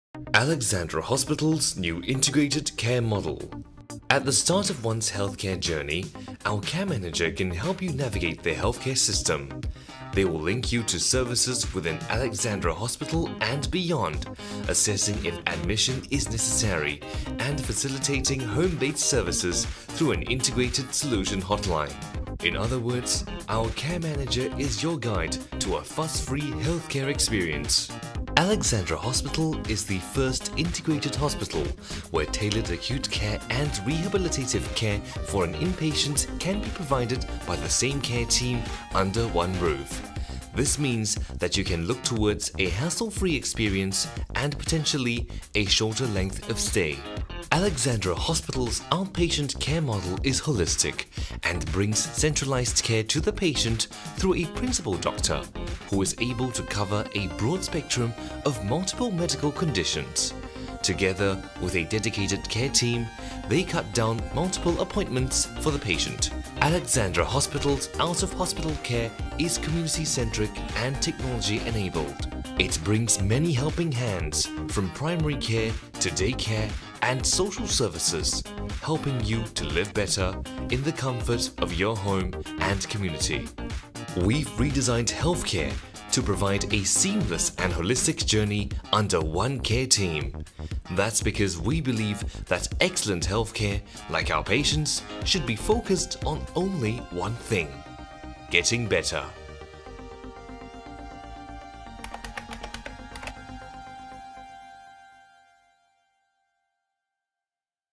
Introducing the Alexandra Hospital's new integrated model of care
The model of health and care at Alexandra Hospital is OOOOOH (One patient, One bed, One care team, One principal doctor, One with the community, Healthcare redesign).  We are Singapore’s first Integrated General Hospital which provides a single continuum of care across different settings from acute, sub-acute to rehabilitative care in the same ward and bed. Care is consolidated by a multidisciplinary team with a principal doctor, and provided through five key programmes. Through the Virtual Care Centre at AH, our care managers will work with patients, families and community partners to anchor care of every discharged patient at home as much as possible and reduce re-admissions.
for more information.

Follow us on on the AH YouTube channel to view the Chinese, Malay and Tamil versions of what AH’s care model is about.